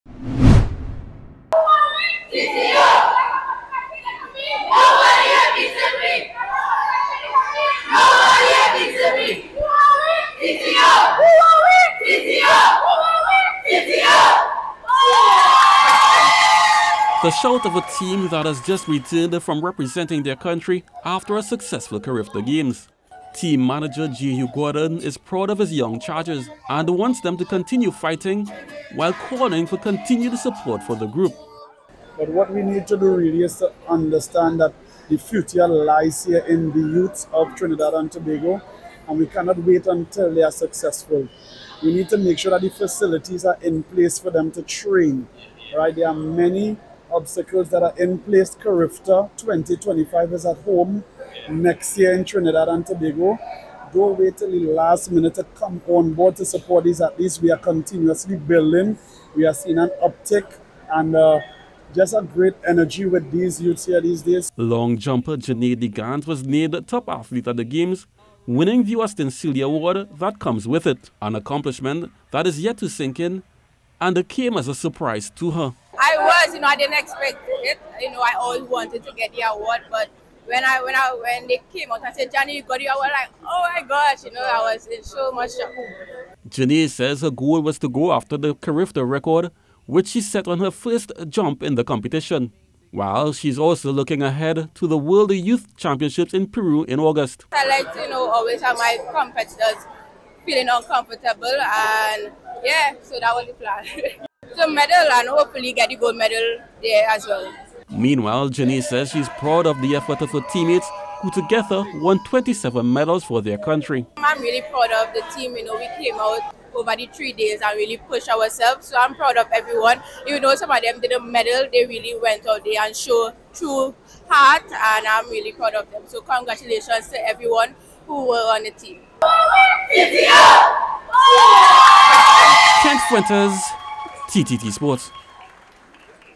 Jubilant CARIFTA Athletes Back Home
The 2024 Carifta Track and Field team returned home from Grenada with 27 medals.

With accomplishments to be proud of, Team Manager, Jehue Gordon, and Janae De Gannes, who was named the Games' best athlete, spoke to the media.